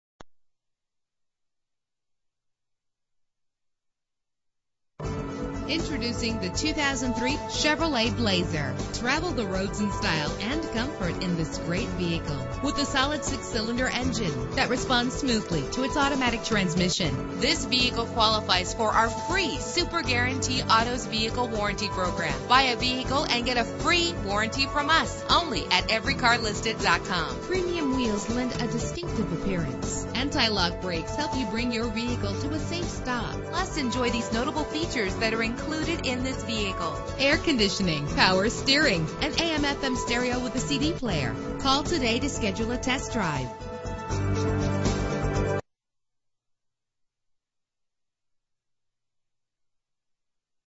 2003 Chevrolet Blazer - Houston TX
Year: 2003
Make: Chevrolet
Model: Blazer
Trim: LS
Engine: 4.3 liter 6 cylinder 12 valve
Transmission: 4-Speed Automatic
Color: Doeskin Tan
Mileage: 50981
Address:
8477 North Freeway